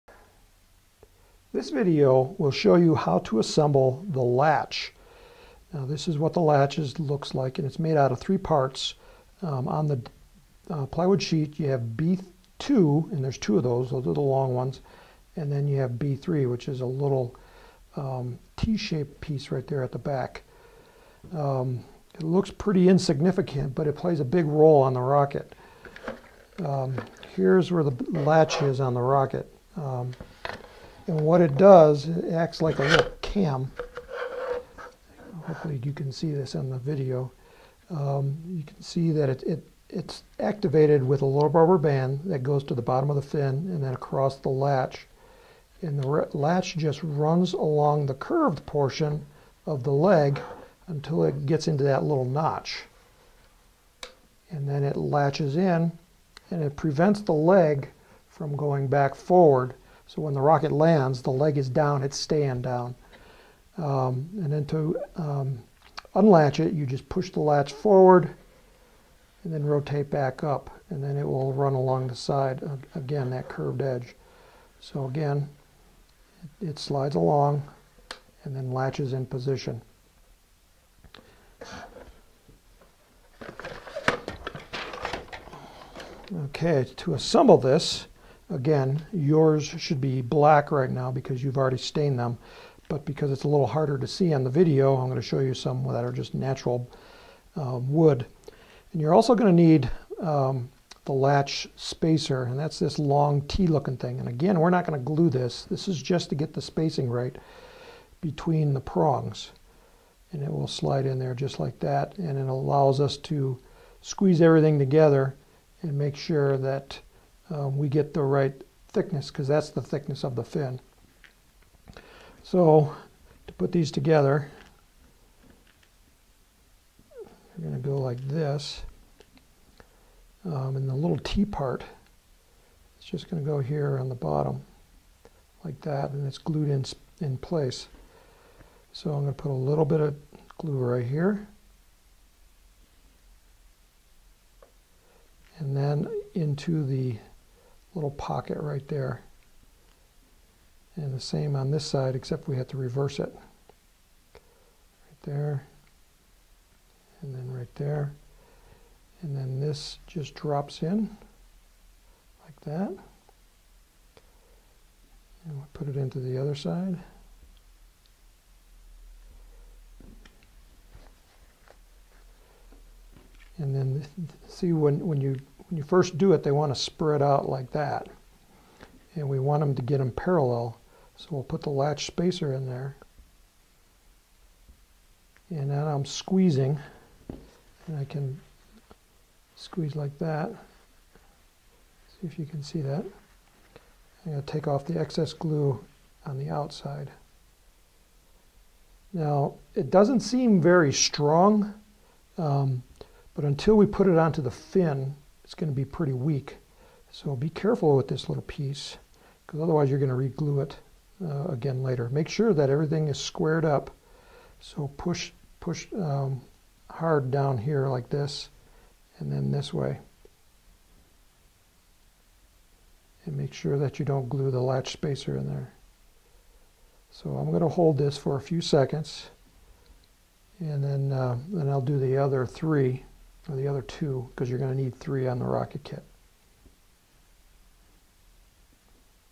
Star Lift Mega Lander Construction 07
- This video covers the assembly of the leg latch piece. The latch engages into the landing legs of the Star Lift Mega Lander, and holds them securely in the deployed position. In other words, it prevents the legs from folding back up when the rocket touches down.